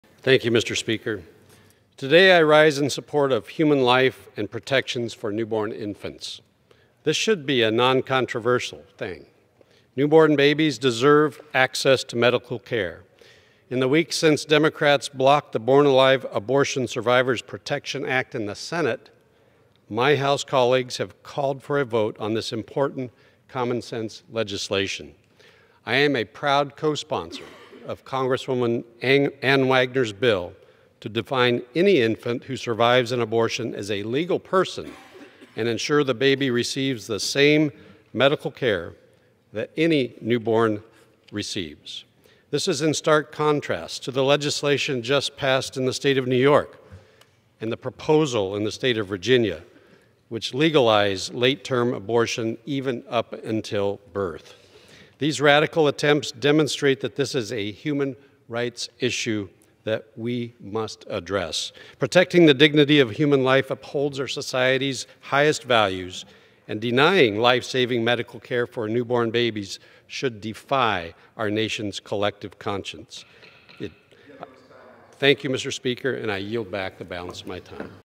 Newhouse Speaks in Support of Life
2/14/2019 Rep. Newhouse (R-WA) speaks on the House floor in support of the Born Alive Abortion Survivors Protection Act.